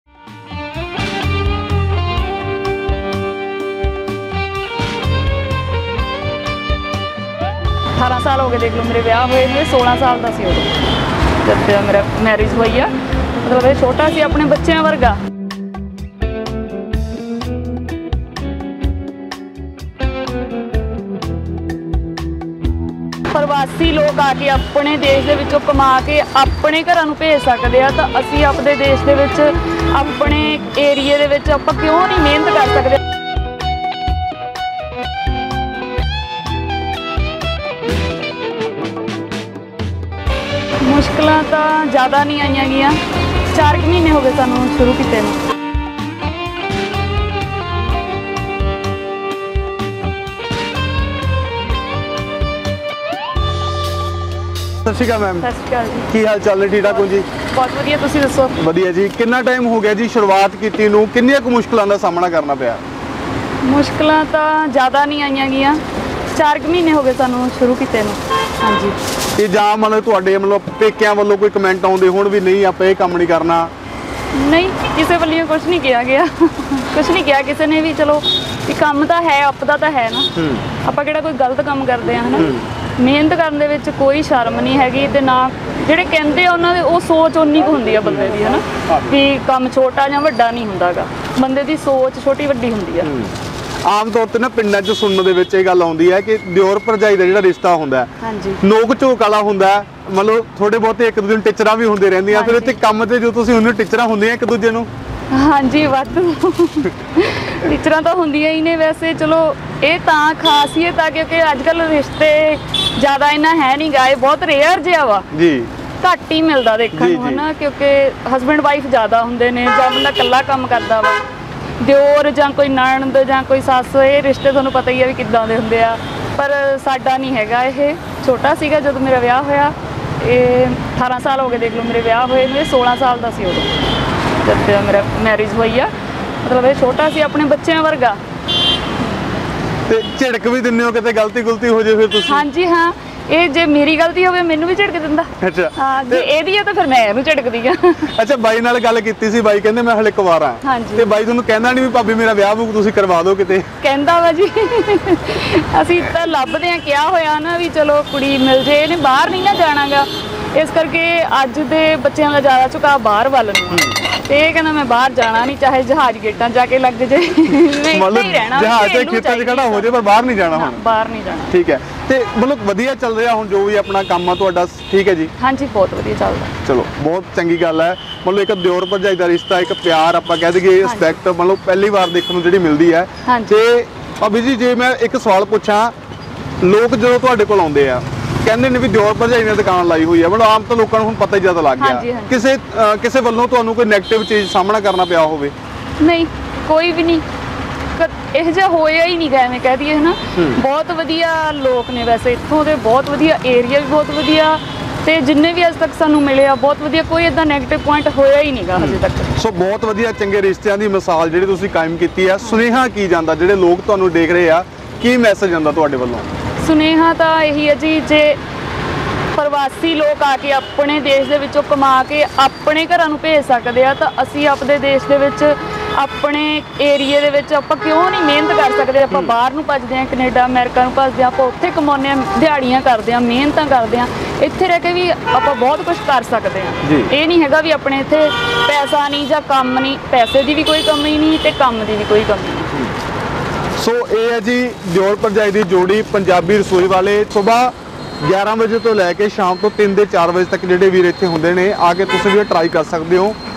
ਇਸ ਦਿਉਰ-ਭਰਜਾਈ ਨੇ ਕੀਤੀ ਅਨੋਖੀ ਮਿਸਾਲ ਕਾਇਮ ਕਹਿੰਦੇ, ਜੇ ਪਰਵਾਸੀ ਇੱਥੇ ਆ ਕੇ ਕਮਾ ਸਕਦੇ ਆ, ਤਾਂ ਅਸੀਂ ਕਿਉਂ
ਇਸ ਦਿਉਰ-ਭਰਜਾਈ ਨੇ  ਕੀਤੀ ਅਨੋਖੀ ਮਿਸਾਲ ਕਾਇਮ ਕਹਿੰਦੇ, ਜੇ ਪਰਵਾਸੀ ਇੱਥੇ ਆ ਕੇ ਕਮਾ ਸਕਦੇ ਆ, ਤਾਂ ਅਸੀਂ ਕਿਉਂ ਨਹੀਂ?